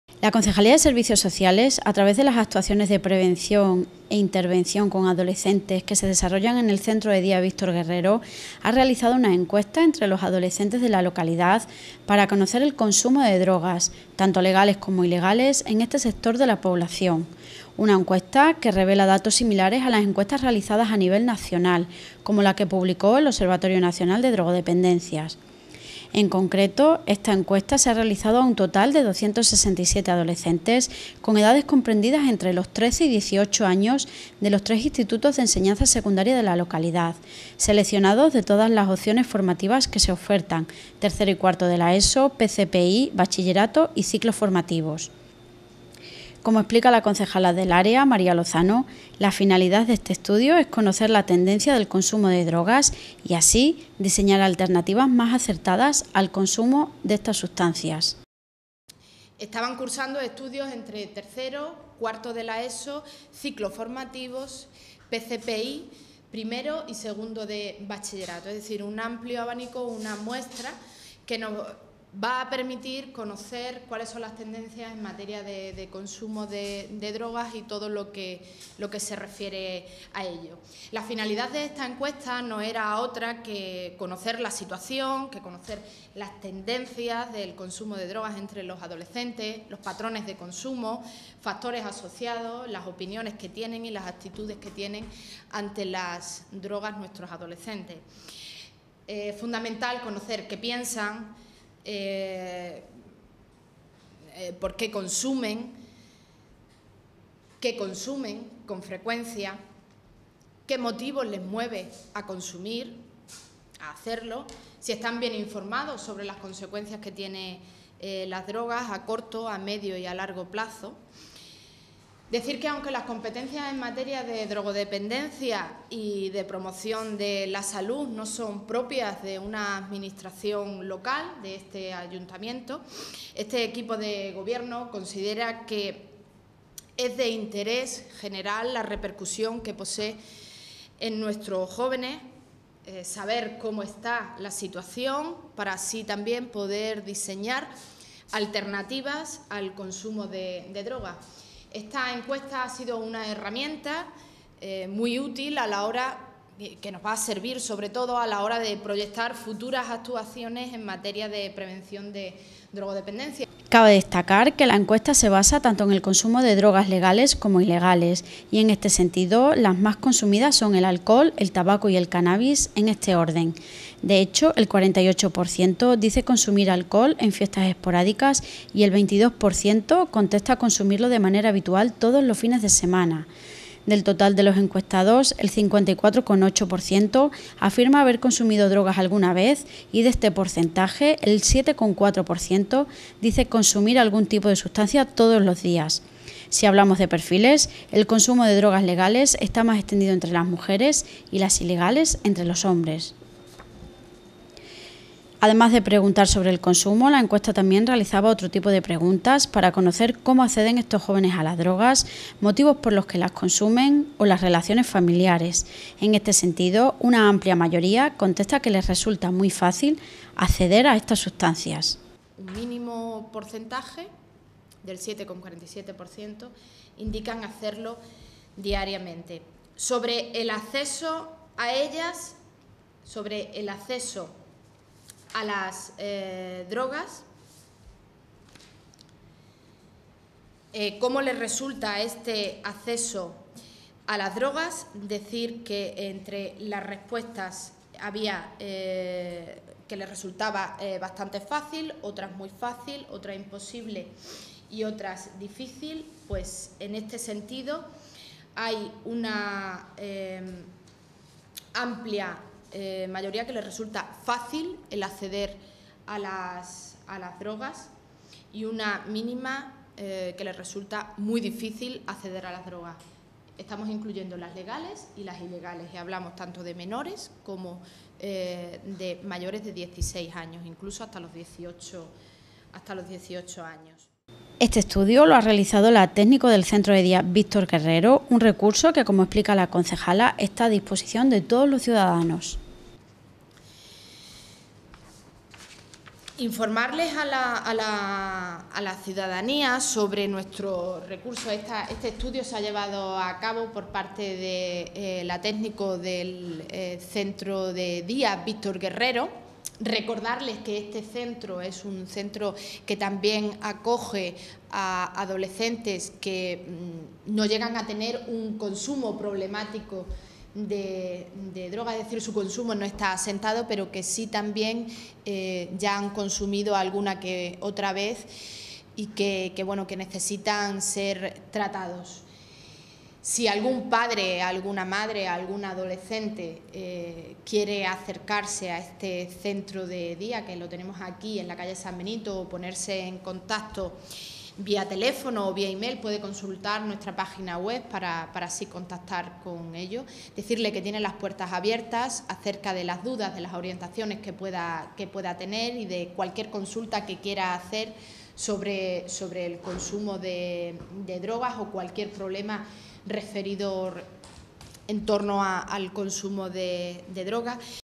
Una encuesta aporta datos sobre el consumo de drogas en la adolescencia
La concejalía de Servicios Sociales, a través de las actuaciones de prevención e intervención con adolescentes que se desarrollan en el centro de día “Víctor Guerrero”, ha realizado una encuesta entre los adolescentes de la localidad para conocer el consumo de drogas, tanto legales como ilegales, en este sector de la población. Una encuesta que revela datos similares a las encuestas realizadas a nivel nacional, como la que publicó el Observatorio Nacional de Drogodependencias.